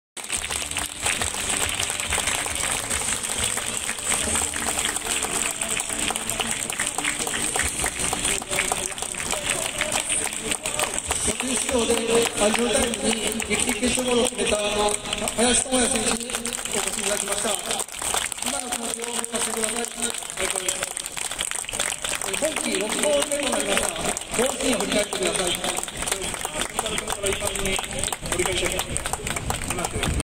2022 J3第20節 ギラヴァンツ北九州：Y.S.C.C.戦終了後、メインスタンドへの選手挨拶
2022年明治安田生命J3リーグ第20節
Y.S.C.C 1-0 ギラヴァンツ北九州
一進一退の攻防も、後半アディショナルタイムにYSCC林にゴールを決められ、勝ち点ゼロ．．．
さすがに選手たち、ショックが大きそう．．．でもまだ、諦めない！